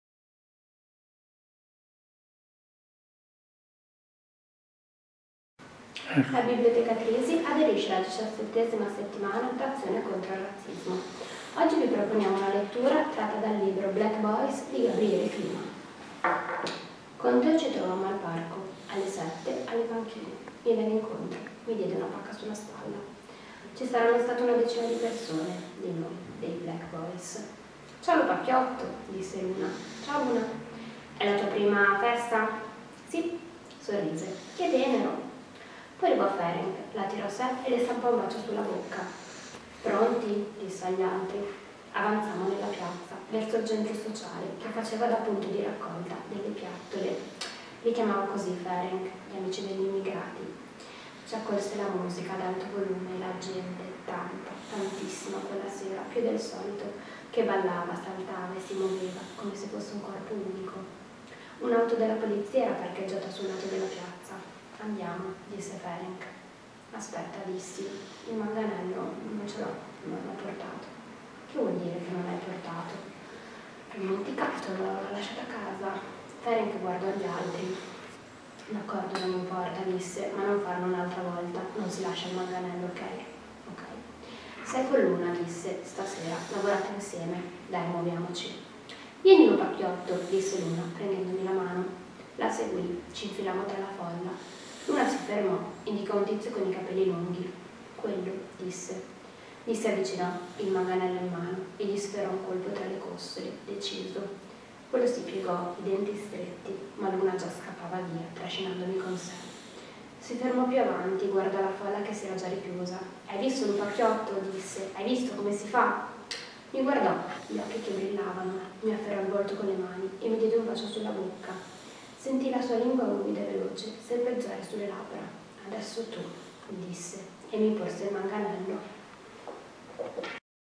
BIBLIOTECHE CONTRO IL RAZZISMO‼️👈 BIBLIOTECA TRISI
Il Centro Antirazzista e sui Rapporti Italia/Sudafrica Centro Documentazione Benny Nato Onlus aderisce alla XVII^ Settimana d’Azione contro il Razzismo – Keep Racism Out (21 - 27 marzo 2021), attraverso il progetto COME PEZZI DI UN PUZZLE - Scuole, biblioteche e associazioni per la promozione di una cultura dell'antidiscriminazione, in collaborazione con Archivio Disarmo - IRIAD - realizzato con il contributo dell’Ufficio Antidiscriminazioni Razziali-Presidenza del Consiglio dei Ministri (UNAR).